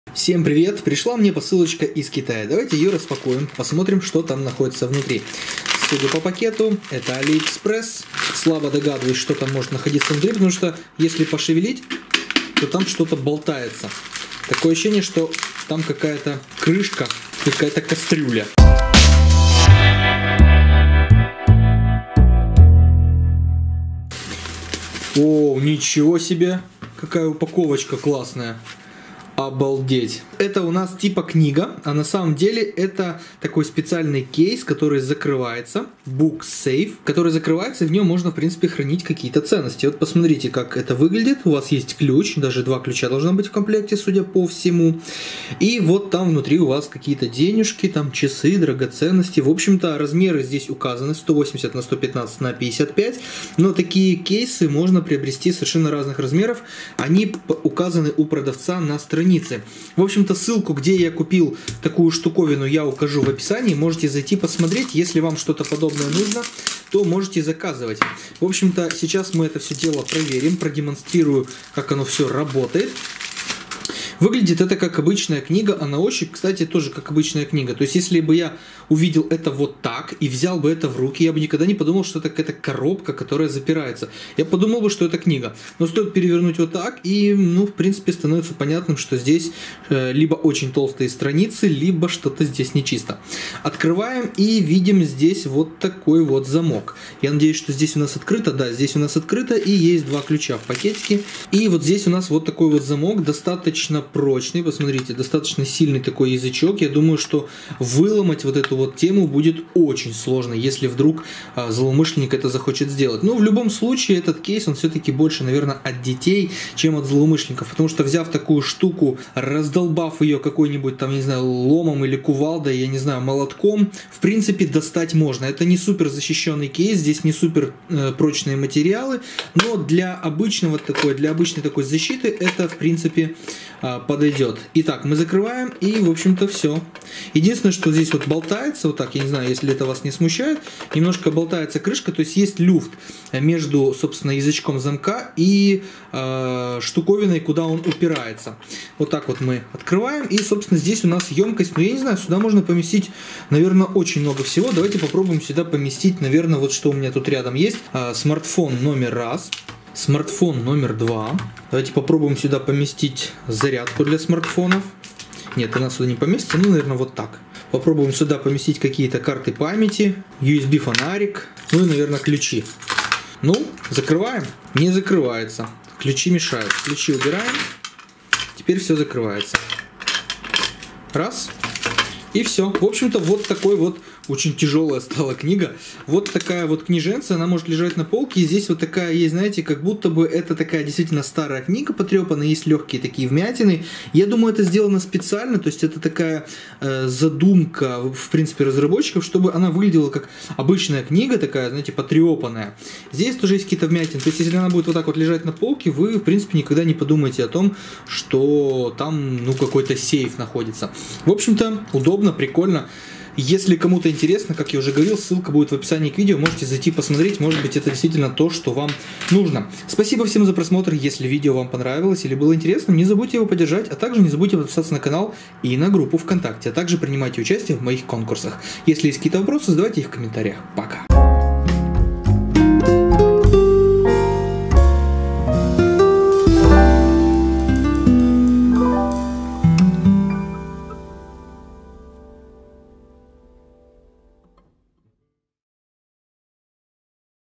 КЕШБОКС КНИГА-СЕЙФ / ГДЕ СПРЯТАТЬ ДЕНЬГИ и ДРАГОЦЕННОСТИ ► Посылка из Китая / AliExpress
От серьезных воров этот КЕШБОКС не спасет, разве только это будет дилетант :) А вот от любопытных детей, особенно маленьких, такая КНИГА-СЕЙФ спасет точно! Качество материалов прочное. Два ключа в комплекте. Короче девайс достойный!